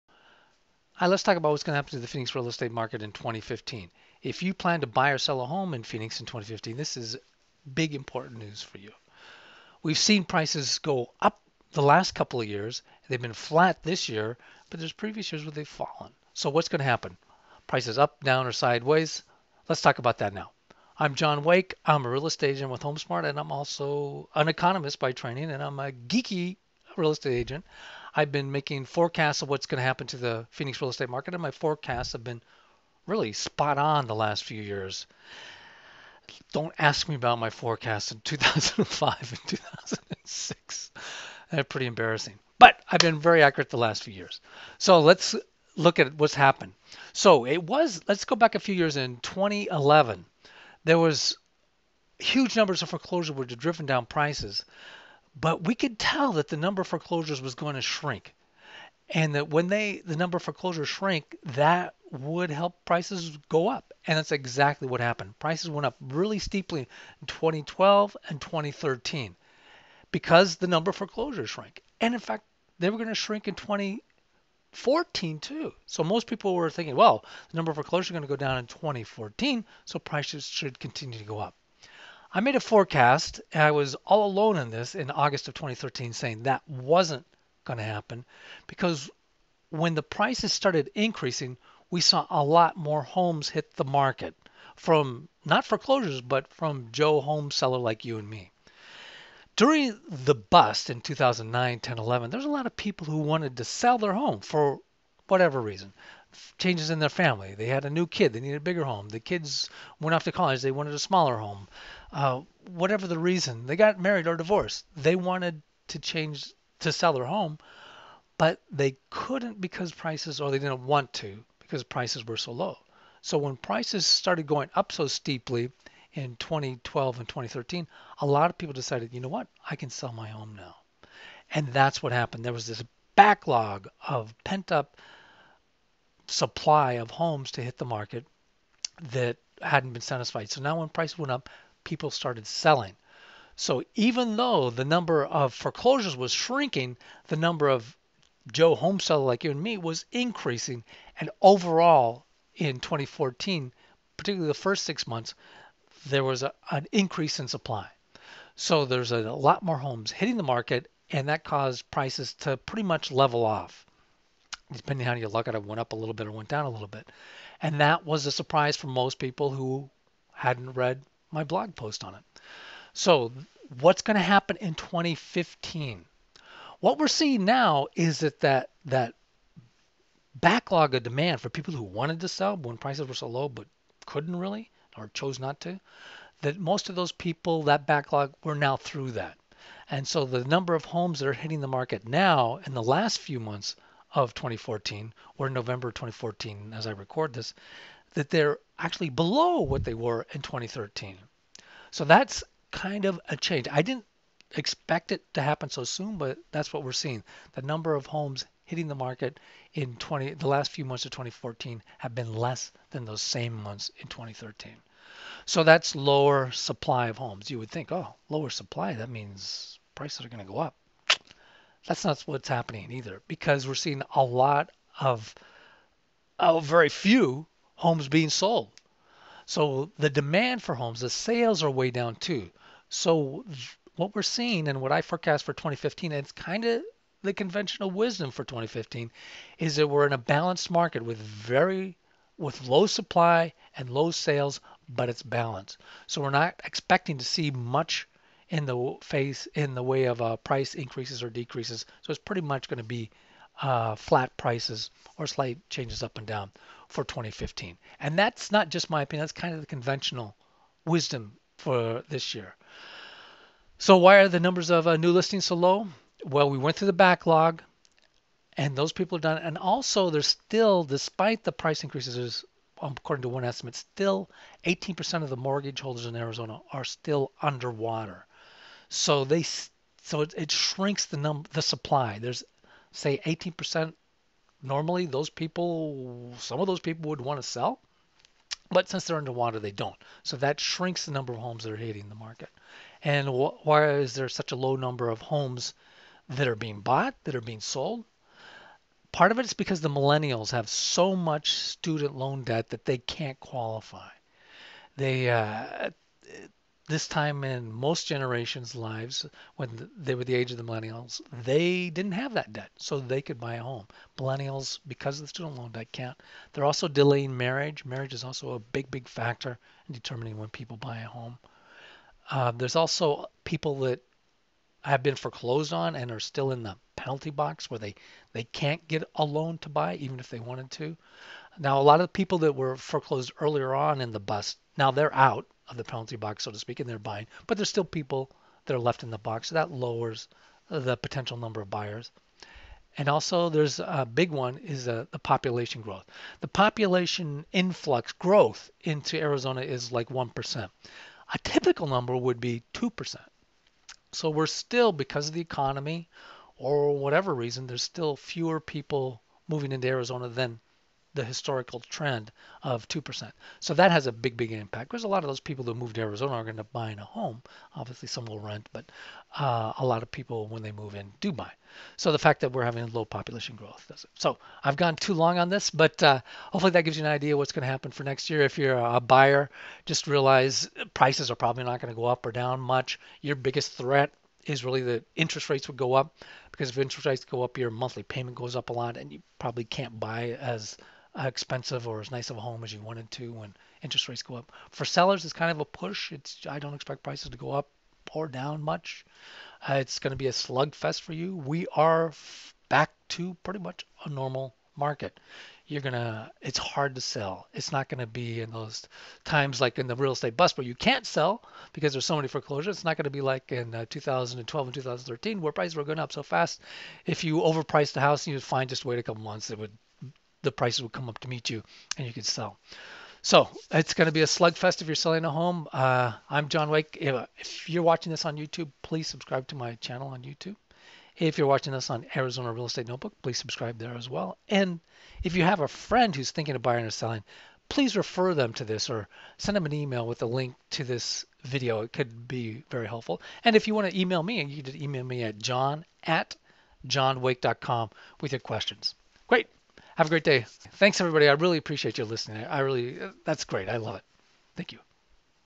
2015 Forecast for Phoenix Real Estate Market and Home Prices - Nov 2014
John Wake of HomeSmart expects 2015 to be the most "normal" real estate market Phoenix has seen in over 10 years with generally flat home prices, a low level of new listings paired with low sales to create a more or less balanced Phoenix residential real estate market.

John Wake
Realtor, Associate Broker, M.S.

SEE ALSO Elliot Pollack's forecast for the Phoenix real estate market in 2015